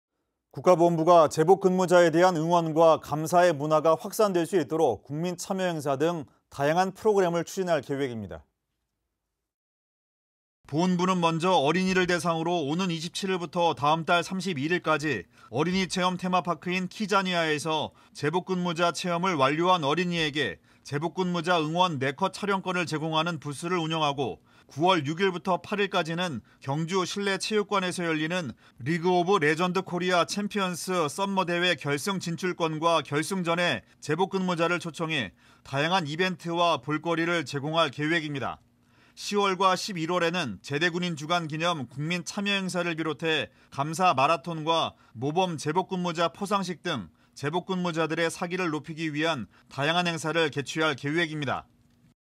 보훈부, 제복근무자 응원 확산 프로그램 [KFN]
국가보훈부가 제복근무자에 대한 응원과 감사의 문화가 확산될 수 있도록 국민 참여 행사 등 다양한 프로그램을 추진할 계획입니다.

보훈부는 먼저 어린이를 대상으로 오는 27일부터 다음 달 31일까지 어린이체험 테마파크인 키자니아에서 제복근무자 체험을 완료한 어린이에게 제복근무자 응원 네컷 촬영권을 제공하는 부스를 운영하고

9월 6일부터 8일까지는 경주 실내체육관에서 열리는 리그오보 레전드 코리아 챔피언스 썸머 대회 결승 진출전과 결승전에 제복 근무자를 초청해 다양한 이벤트와 볼거리를 제공할 계획입니다.

10월과 11월에는 제대군인주간 기념 국민 참여행사를 비롯해 감사 마라톤과 모범 제복근무자 포상식 등 제복근무자의 사기를 높이기 위한 다양한 행사를 개최할 계획입니다.